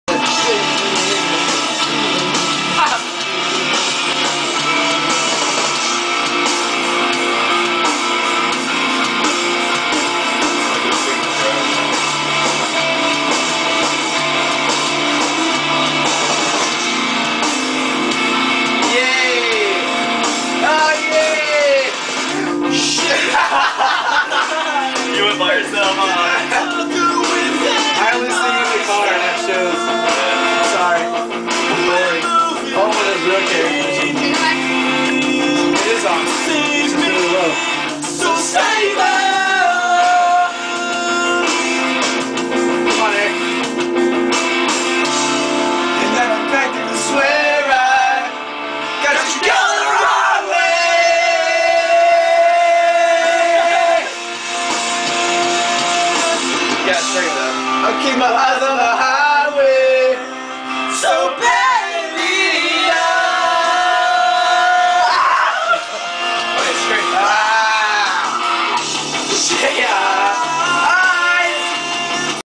boys sing at hollys(:
three boys if in our time ( eric, josh, and dubee) sing at hollys house(: